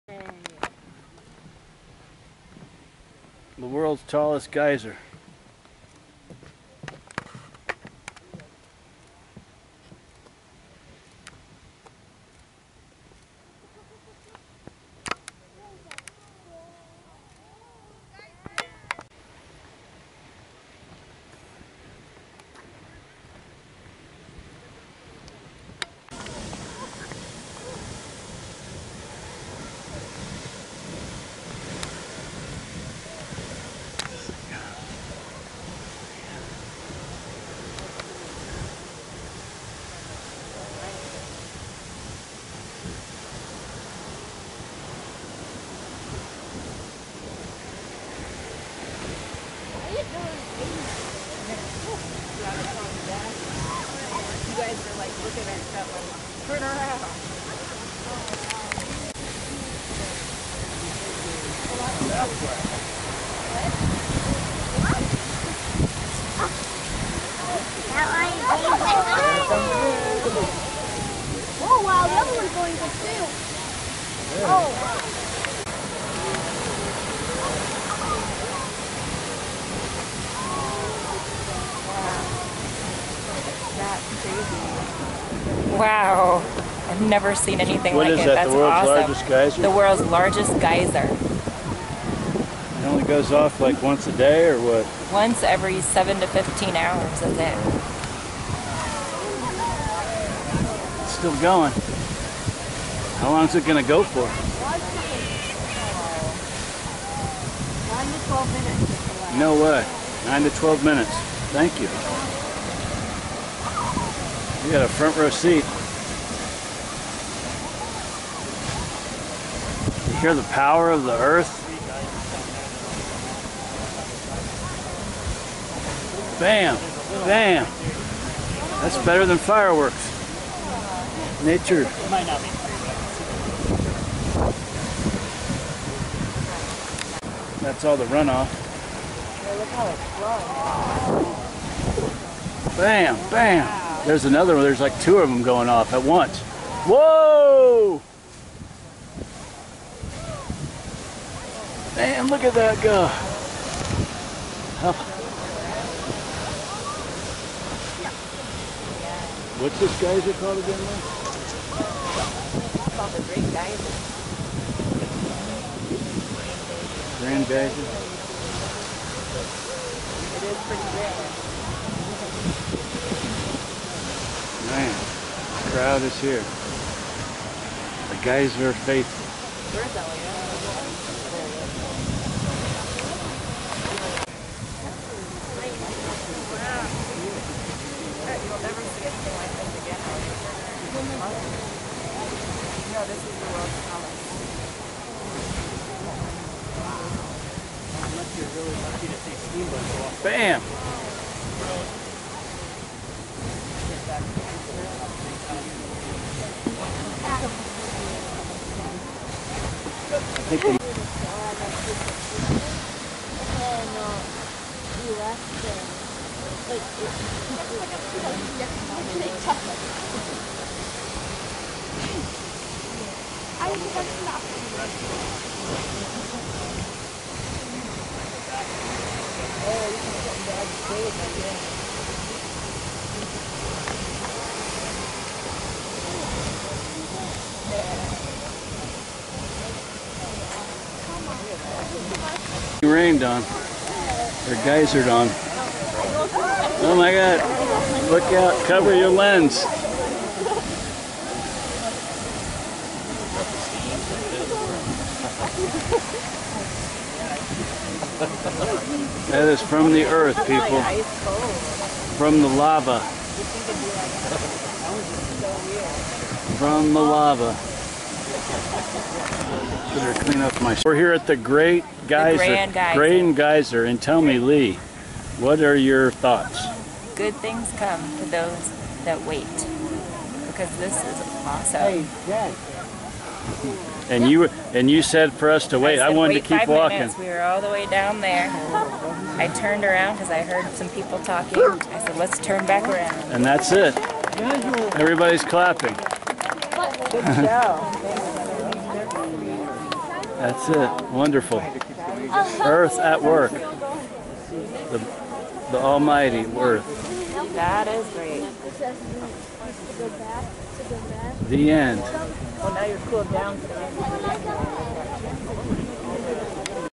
Yellowstone Grand Geyser Goes Off As We Walk by
We walked by the grand geyser and noticed a mormot standing on the hillside like he new something was going to happen. Then suddenly the Geyser blew.